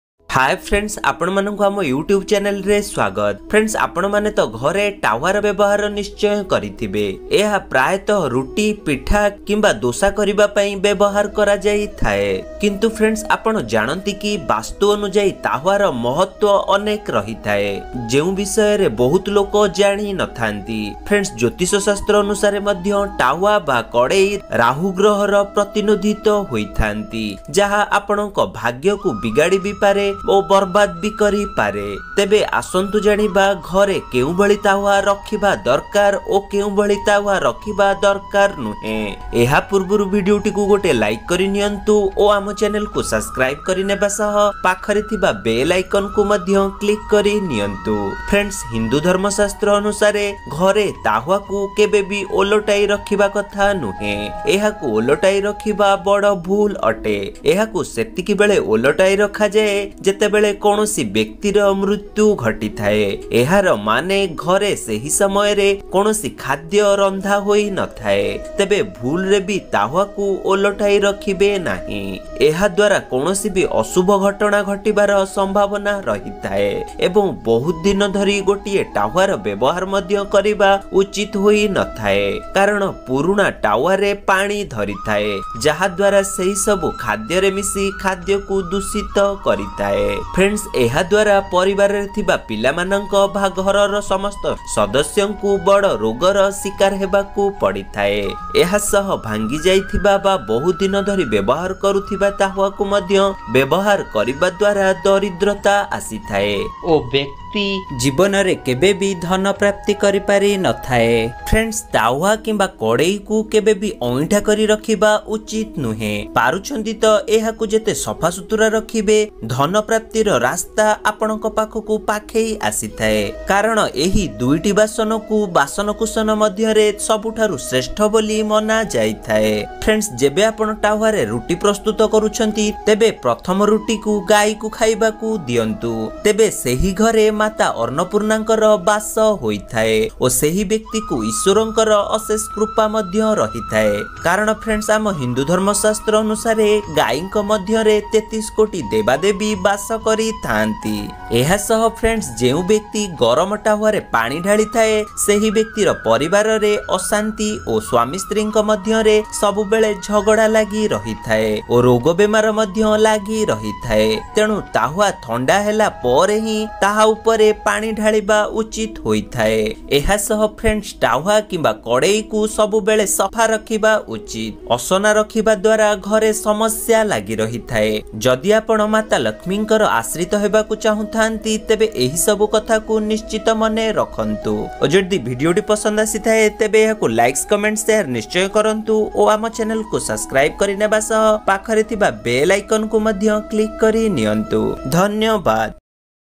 ରୋଷେଇ ଘରେ କଢ଼େଇ ସହ କରନ୍ତୁ ଏହି ବିଶେଷ ଉପାୟ ମା ଲଷ୍ମୀ ହେବେ ପ୍ରସନ୍ନ, କରିବେ ଧନବର୍ଷା- Vastu Tips
Vastu Sastra || ମରିଗଲେବି ଏପରି ଟାଓ୍ବାରେ କେବେବି ରୁଟି କରନ୍ତୁ ନାହିଁ, ପୁରା ଘର ନଷ୍ଟ ହୋଇଯାଏ |odia bayagita tips

Hii friends ap na manku ama youtube channel re swagat .. Aji ehi video re ame jnaiba Vasu Sastra anusare ghare keun tawa rakhiba uchit o keun tawa rakhiba anuchit hoithae ..